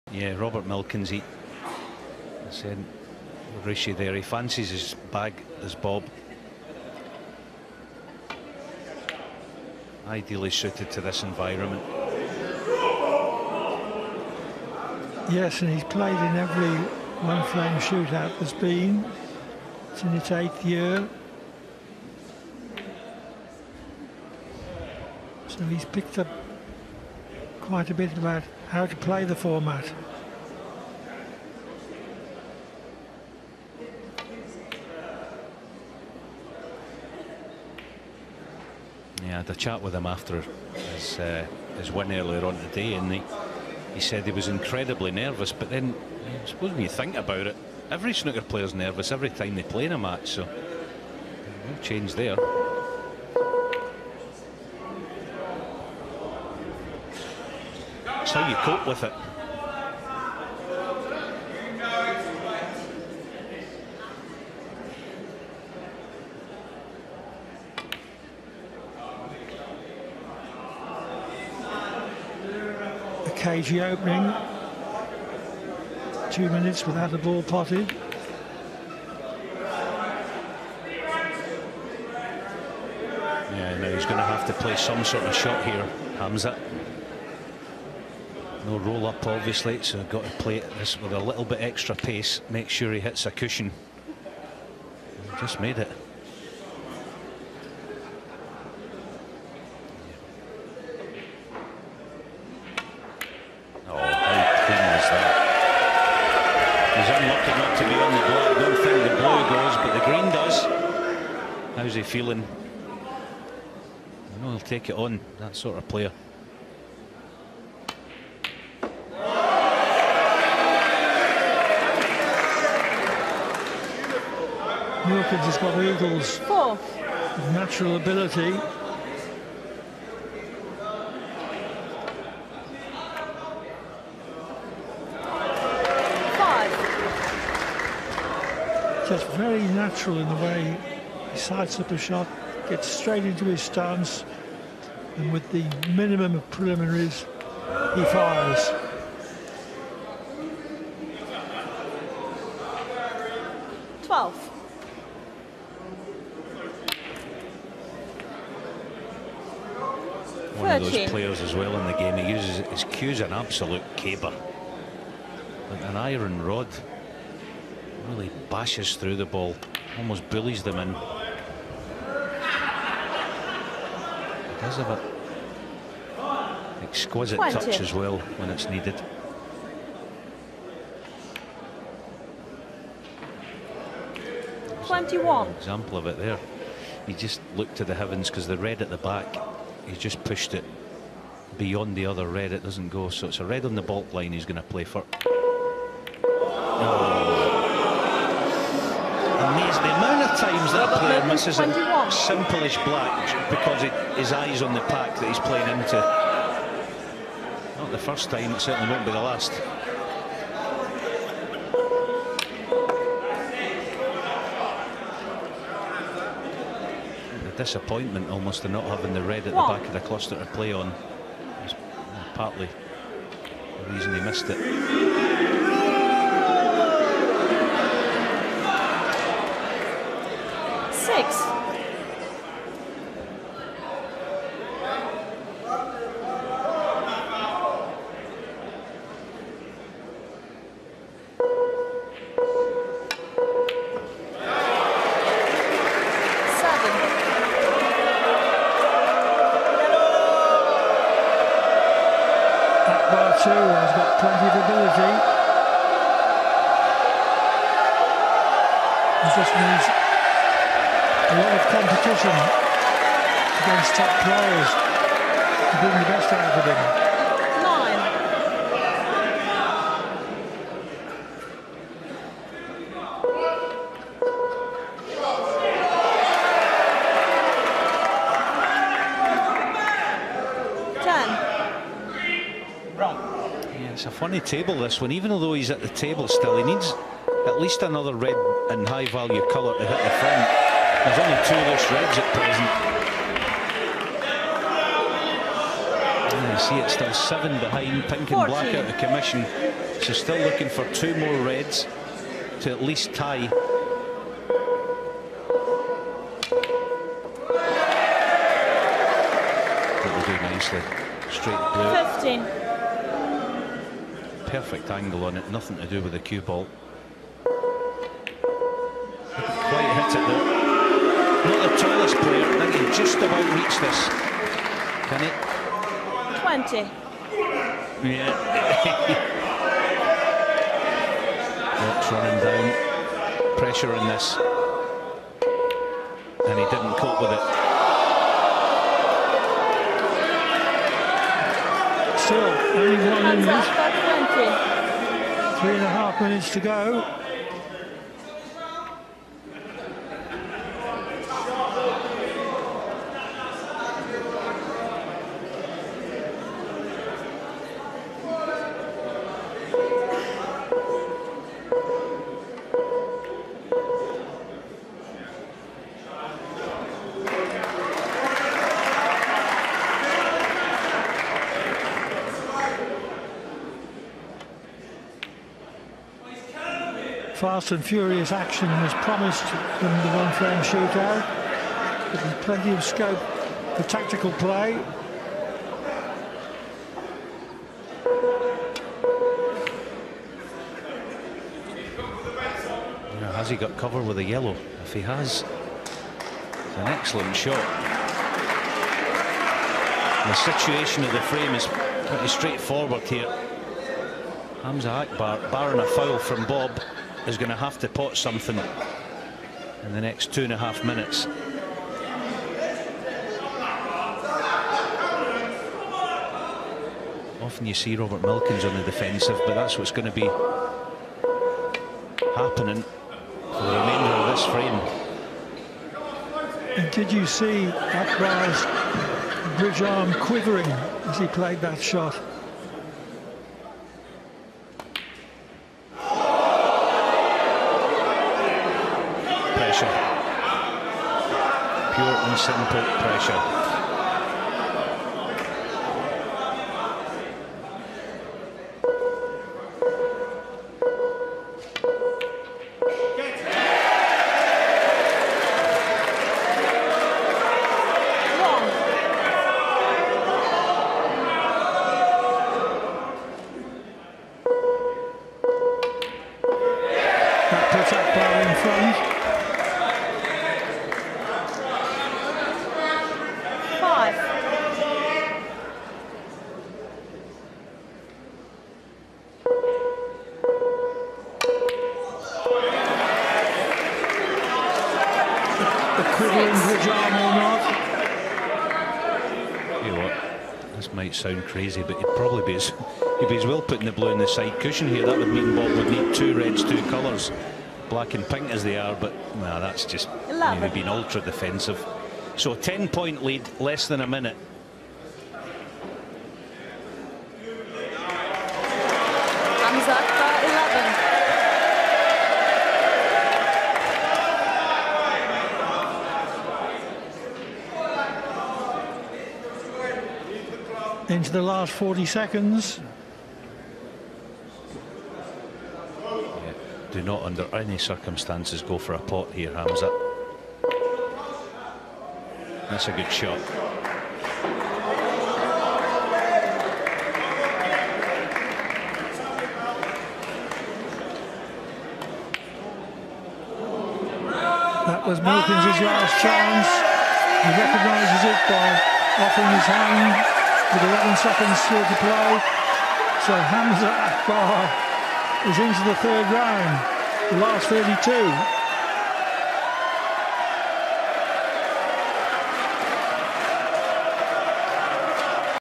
Hamza Akbar Vs Robert Milkin Pakistan vs United kingdom Coralshoot 2018#robert_milkins
Hamza Akbar Vs Robert Milkin (Pakistan vs United kingdom) Coral shoot out 2018

pakistani player